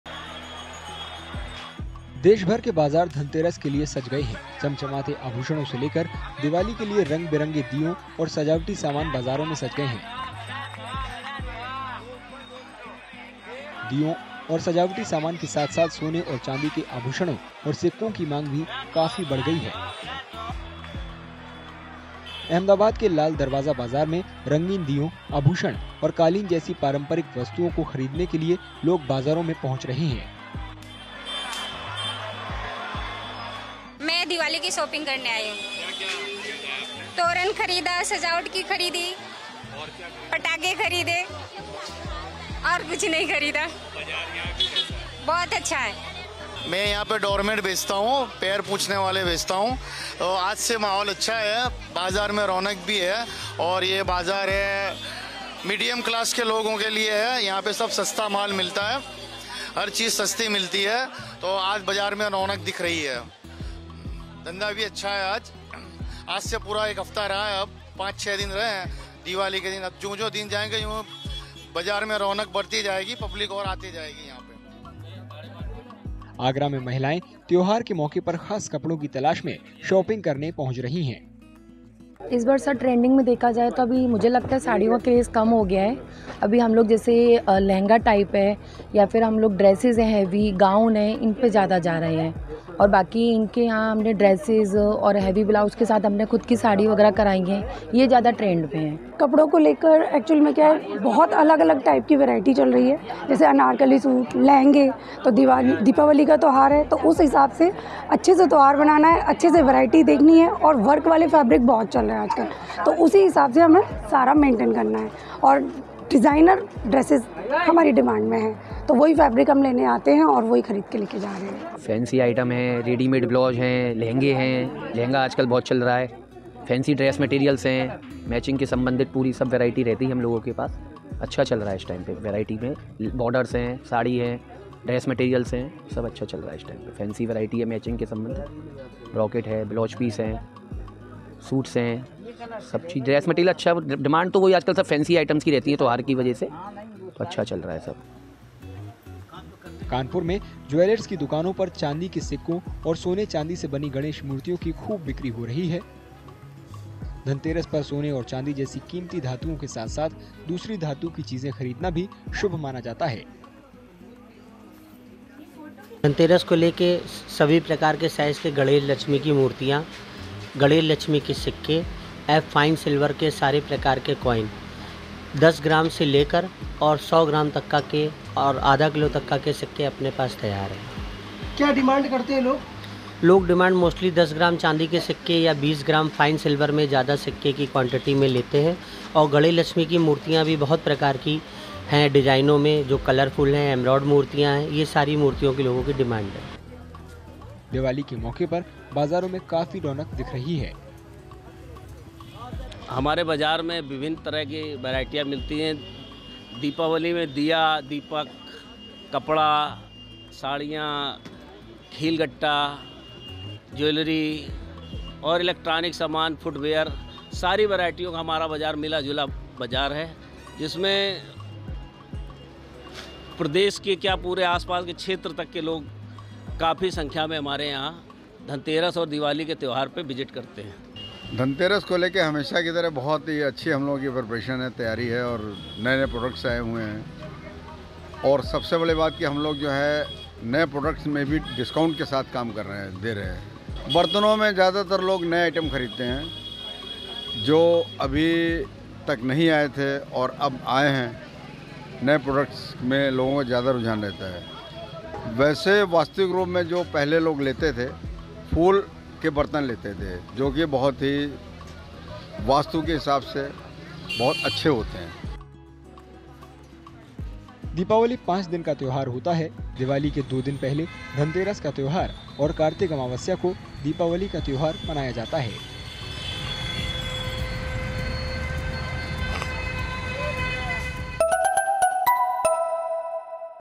दीपावली पर देश भर में सज गए बाजार, सोने-चांदी के सिक्कों और आभूषणों की बढ़ गई है मांग
देश भर के बाजार धनतेरस के लिए सज गए हैं। चमचमाते आभूषणों से लेकर दिवाली के लिए रंग-बिरंगे दीयों और सजावटी सामान बाजारों में सज गए हैं। दीयों और सजावटी सामान के साथ-साथ सोने और चांदी के आभूषणों और सिक्कों की मांग भी काफी बढ़ गई है। अहमदाबाद के लाल दरवाजा बाजार में रंगीन दीयों, आभूषण और कालीन जैसी पारंपरिक वस्तुओं को खरीदने के लिए लोग बाजारों में पहुंच रहे हैं।